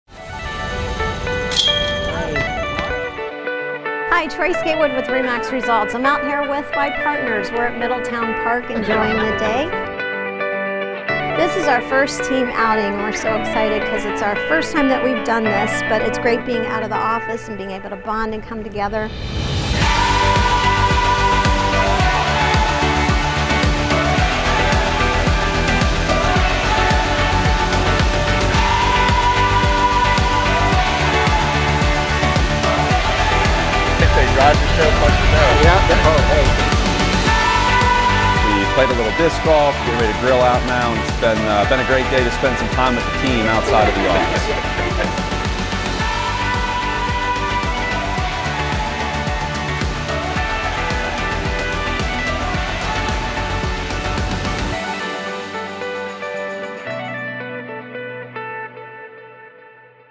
TGP Goes Disc Golfing
Once a month our team has an outing to breakaway from the office and bond together as a team. In September we played Disc Golf and finished off the afternoon with a Potluck. It was a sweet time and we are grateful to have the opportunities to work and enjoy life together.